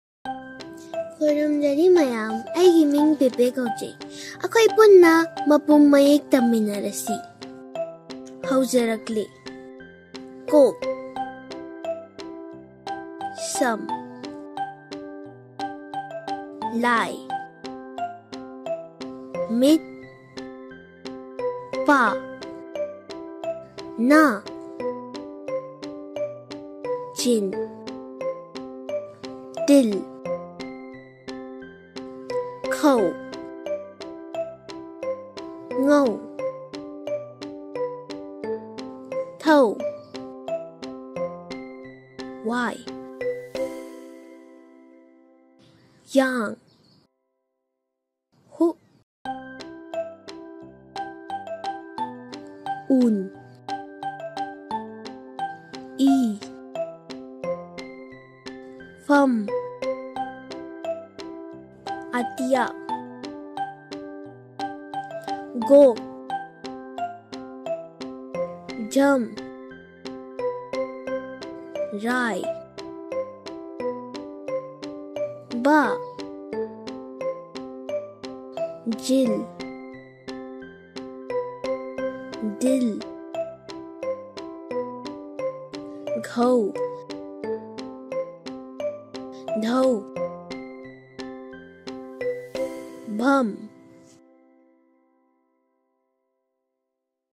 Mapum Mayek (kok sam lai)
Join us on a magical journey to explore the Meitei Mayek script, an enchanting writing system from the heart of Manipur! 🎉📚 In this colorful and exciting video, we'll dive into the fascinating world of Meitei Mayek and  unique symbols.

Our adventure starts with a lively introduction to the script's beautiful characters.
Perfect for kids of all ages, this video makes learning about Meitei Mayek an unforgettable experience. So, grab your crayons and get ready to explore the wonders of this ancient script with us!